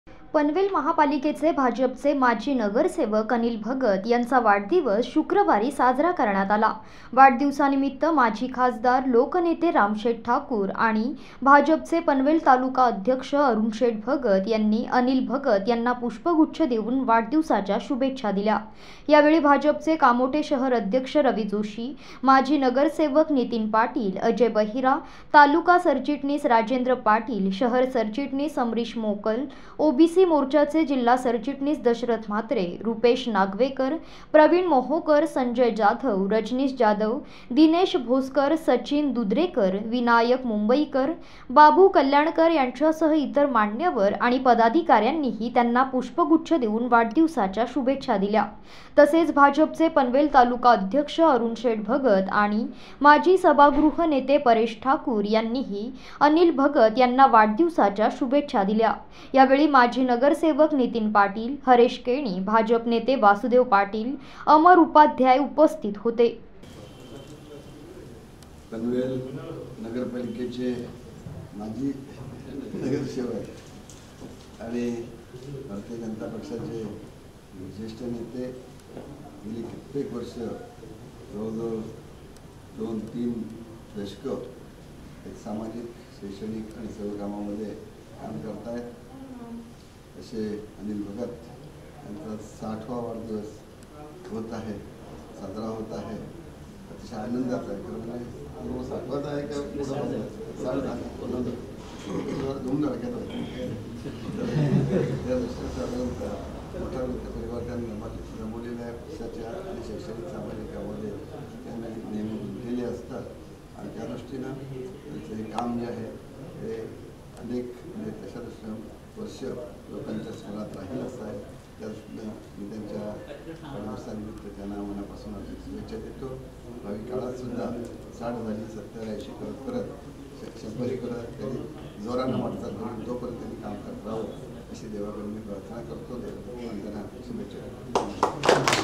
माजी नगरसेवक अनिल भगत यांचा वाढदिवस उत्साहात साजरा !
माजी नगरसेवक अनिल भगत यांचा वाढदिवस उत्साहात साजरा; माजी खासदार लोकनेते रामशेठ ठाकूर यांनी माजी नगरसेवक अनिल भगत यांना वाढदिवसाच्या दिल्या शुभेच्छा !malharnews